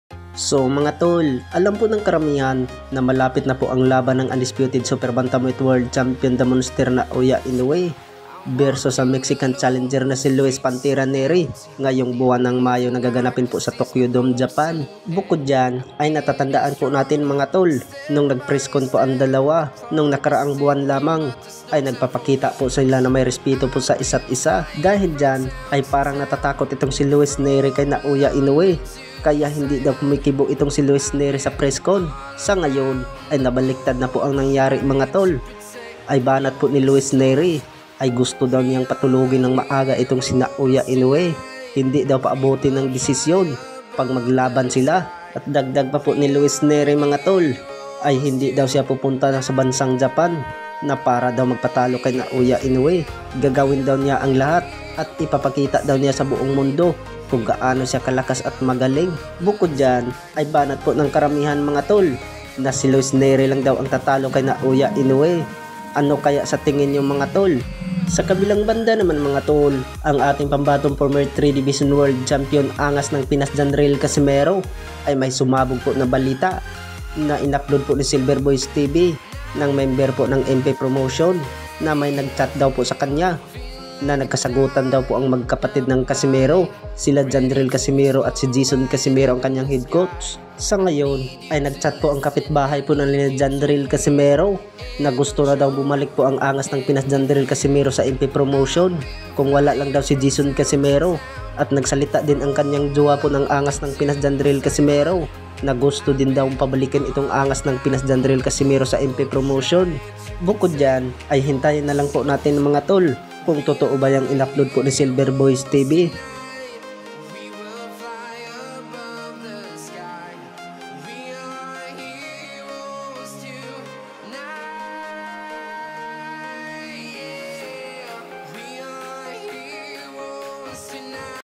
Breaking news!John riel casimero gusto na bumalik sa MP PROMOTION|INOUE vs nery may BANAT
Breaking news updates! John riel casimero gusto bumalik sa MP PROMOTION|Naoya INOUE vs Luis nery boxing latest news updates